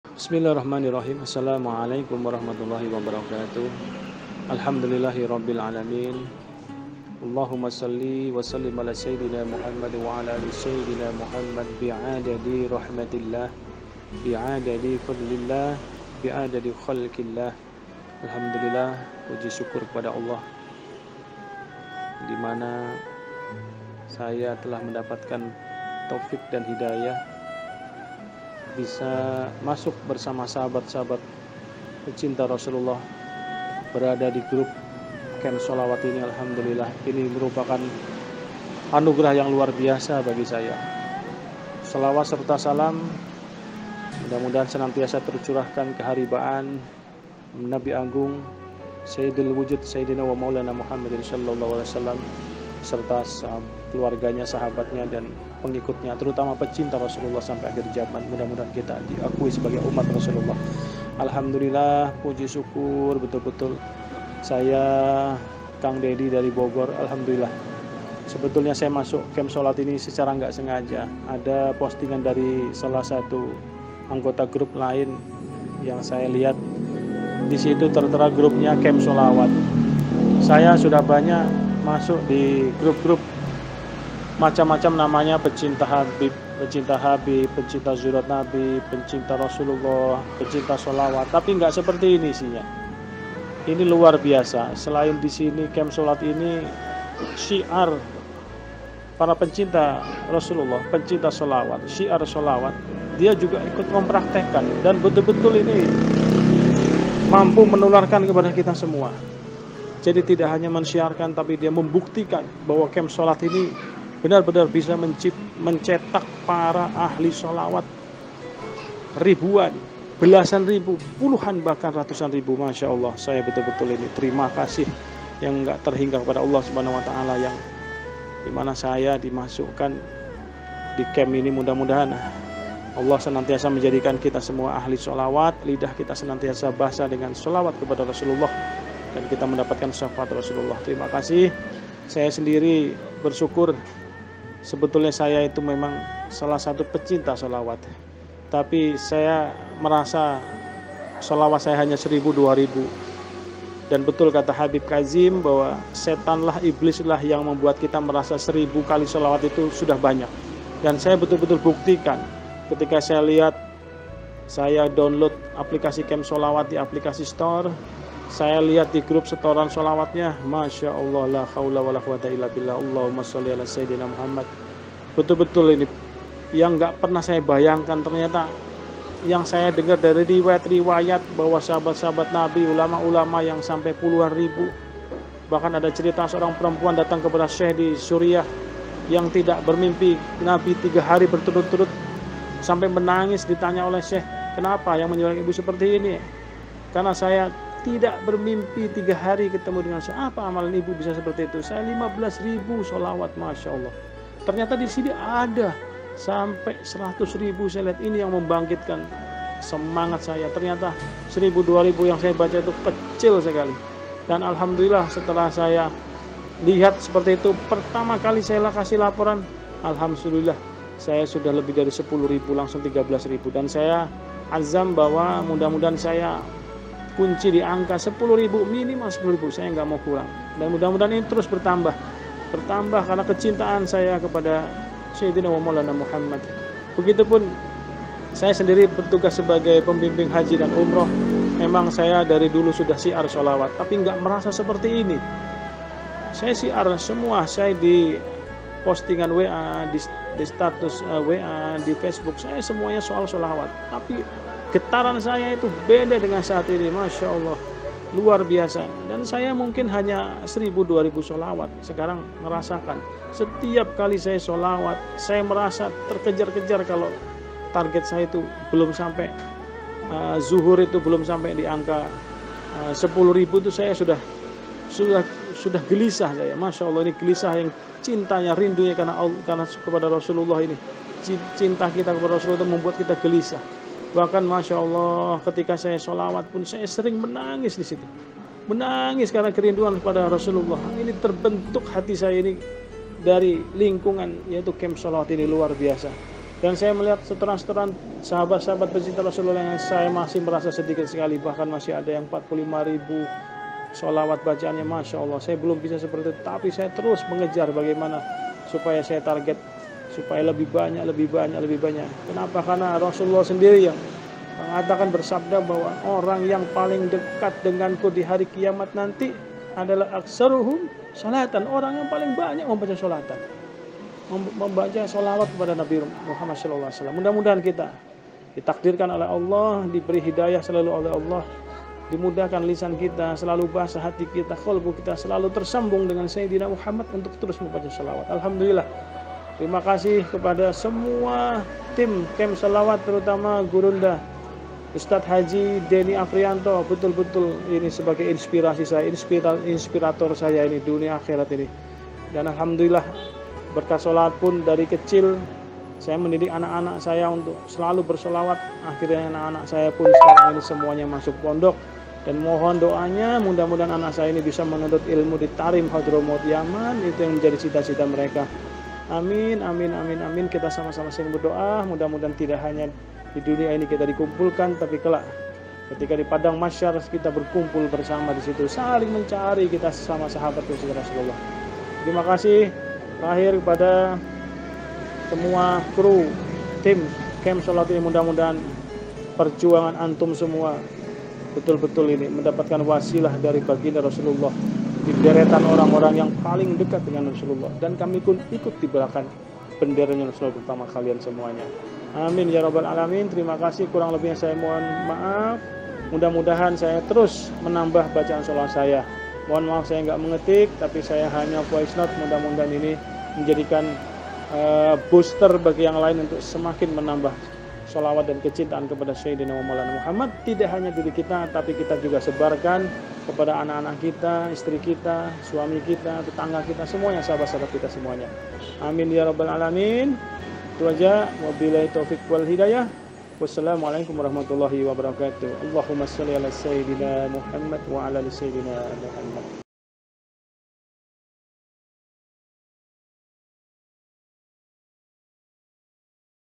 Camp Sholawat anugerah buat saya dan umat, hingga kami bisa sholawat puluhan ribu
Ingin bisa sholawat ribuan setiap hari dengan ringan dan nikmat, silahkan hubungi koordinator Camp Sholawat di Berbagai Negara, kik